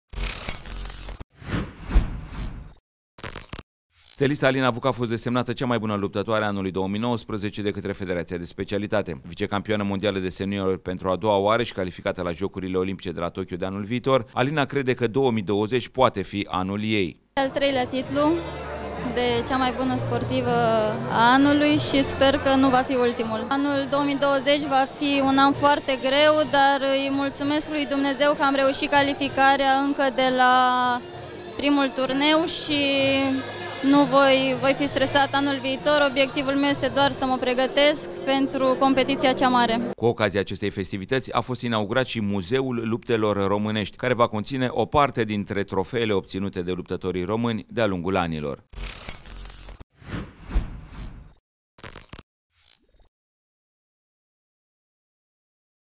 Alina VUC, sportiva anului la FR Lupte
WEB: CSA STEAUA BUCURESTI

Stelista Alina Vuc a fost desemnata cea mai buna luptatoare a anului 2019 de catre Federatia de specialitate. Vicecampioana mondiala de seniori pentru a doua oara si calificata la Jocurile Olimpice de la Tokyo de anul viitor Alina crede ca 2020 poate fi anul ei.

Cu ocazia acestei festivitati a fost inaugurat si Muzeul luptelor romanesti care va contine o parte dintre trofeele obtinute de luptatorii romani de-a lungul anilor.